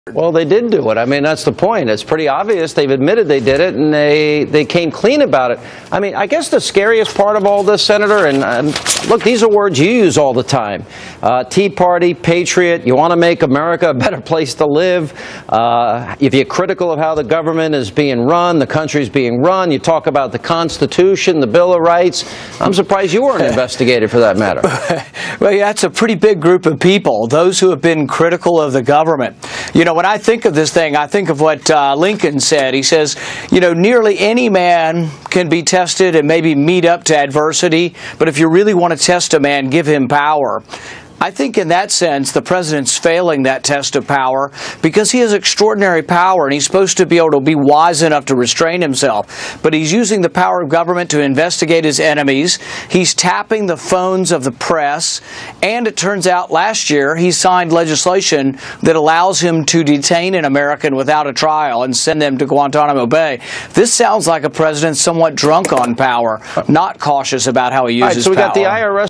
Rand Paul: Obama is drunk on power
Rand Paul talked about the 3 scandals facing Obama. Obama has tremendous power and is using the power to investigate journalists, conservative groups, and sign legislation to send Americans to Gitmo. Paul said that Obama is drunk on power.